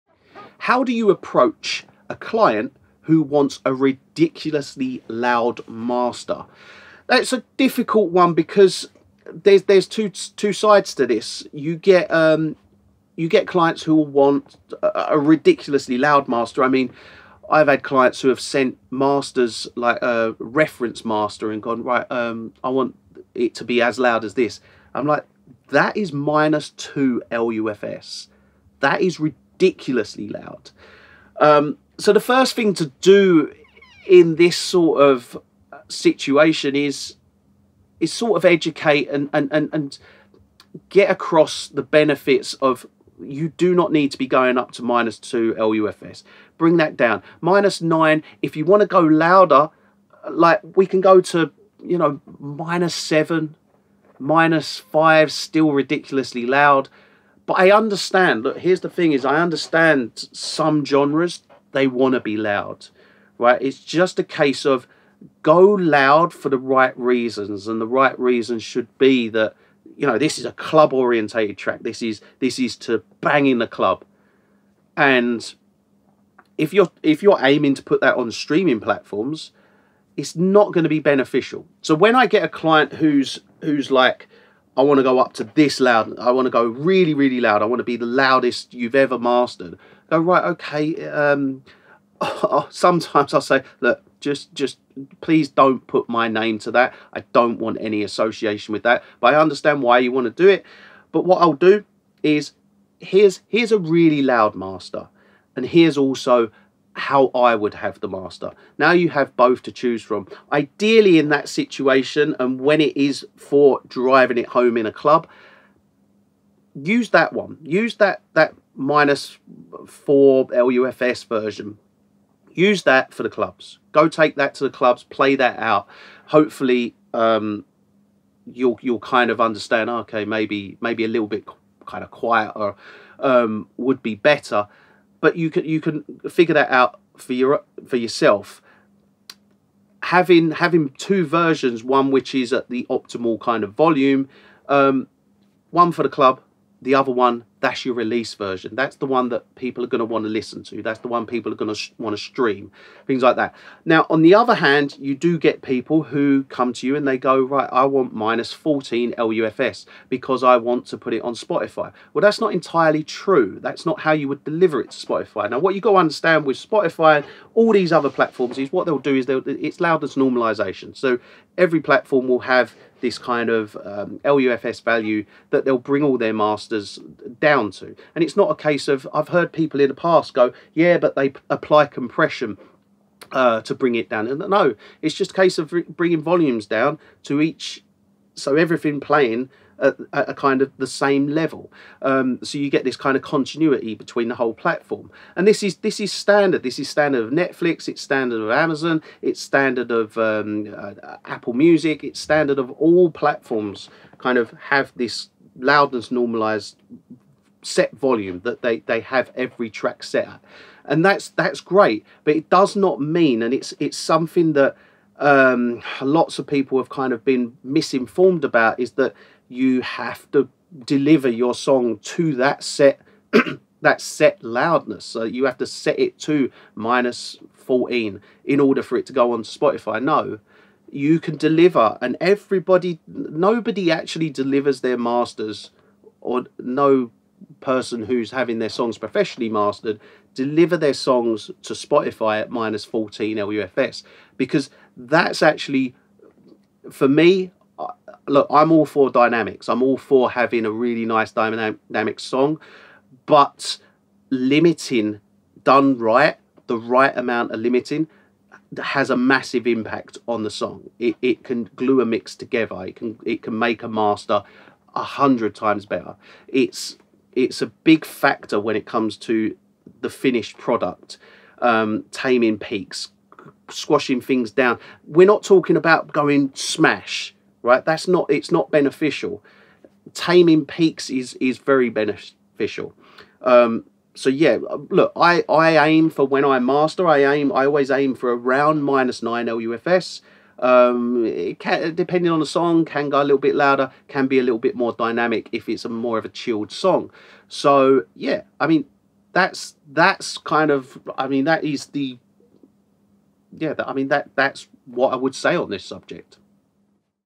How Do You Approach A Client That Wants A Ridiculously Loud Master?
In this video I discuss a sensitive subject on the loudness war we as mastering engineers have to face on a daily basis. Some clients will request a crazy loud master that will have a negative effect on the overall quality of the song. Whilst I find this crazy and ridiculous I do understand why some clients want this kind of loudness when mastering.